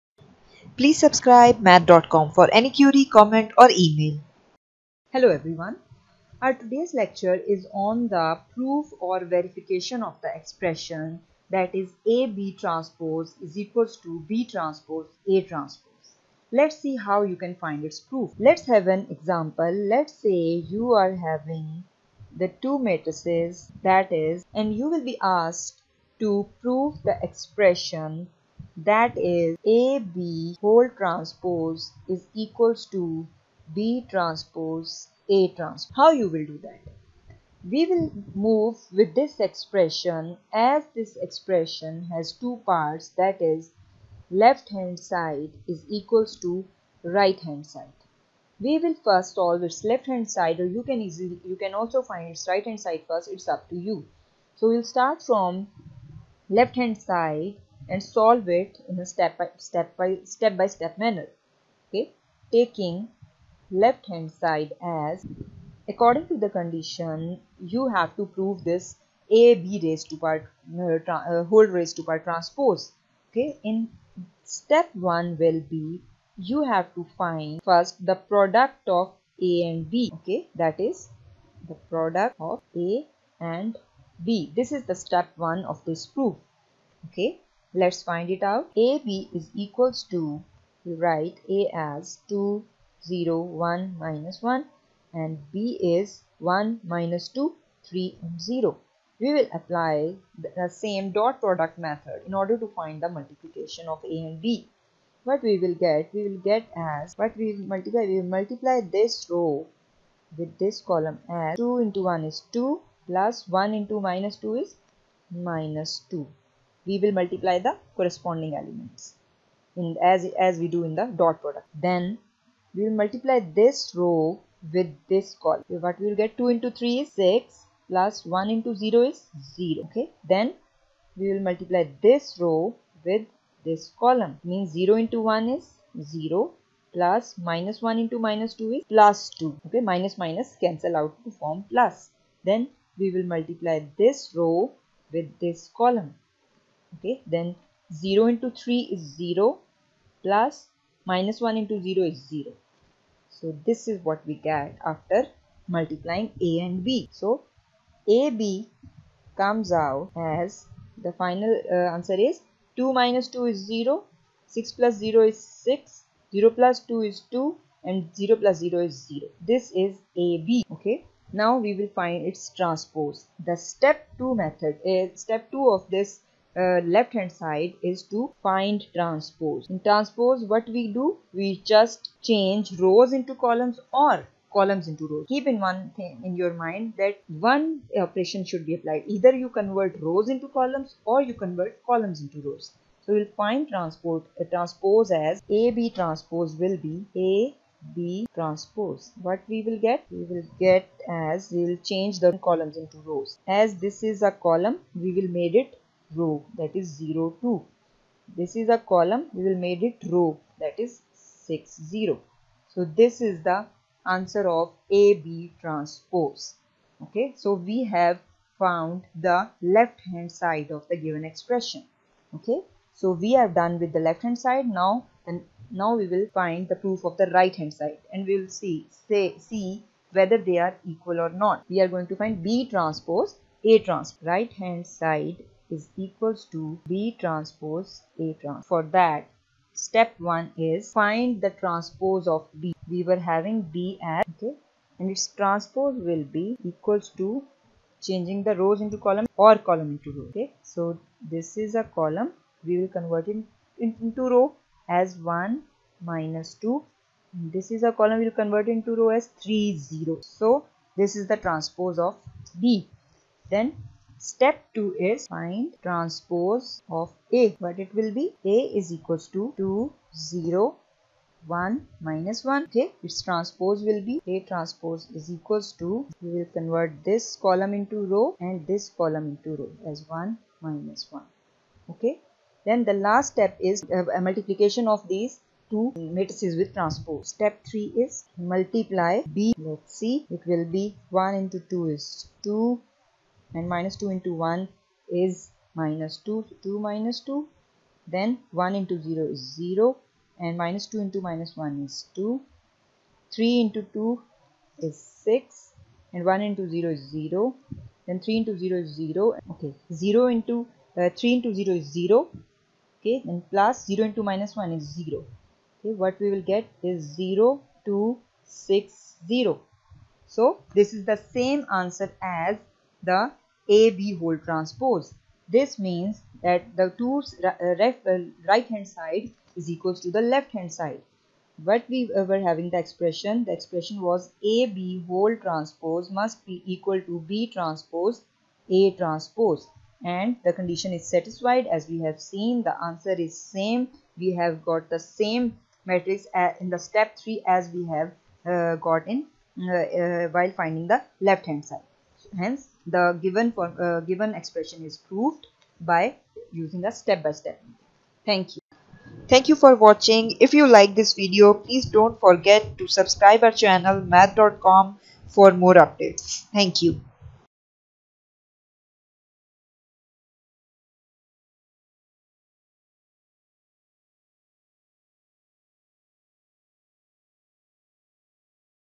Verify (AB)^t = B^t A^t Using Example | Math Dot Com
A step by step verification of the expression (AB)t = Bt At is explained in this video.Take L.H.S of the expression and solve step by step:
Step 1: Find the product of two matrices A and B.
Step 2: Take the transpose of the product of two matrices.
Now take R.H.S and solve step by step:
Step 1: Find transpose of Matrix B.
Step 2: Find transpose of Matrix A.
Step 3: Find the product of step 1 and step 2 matrices.
Compare the L.H.S with R.H.S and check if they are equal.